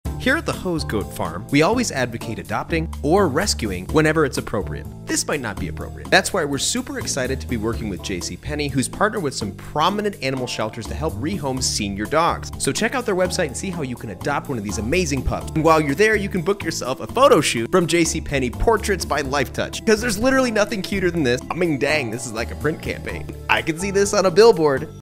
Esperborzoi's Day at JCPenney Portrait Studio
This is your sign to adopt 👆and if you have a fur bestie, come take some pictures with them at our Portrait Studio. Meet these adobrable pups now

Esperborzoi takes pet portraits at JCPenney Portrait Studio.